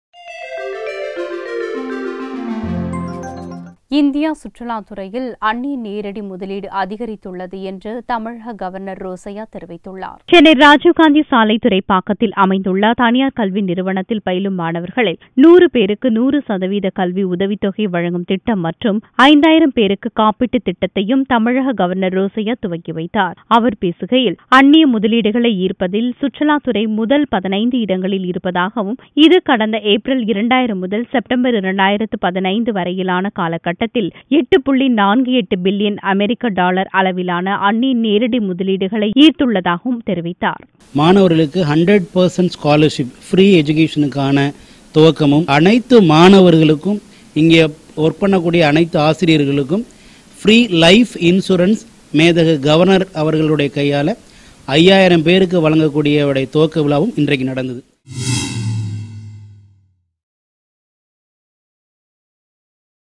Foreign direct investment to Indian tourism has gradually increased : Governor Rosaiah
Foreign direct investment to Indian tourism has gradually increased : Governor Rosaiah - Dinamalar July 6th 2016 For more videos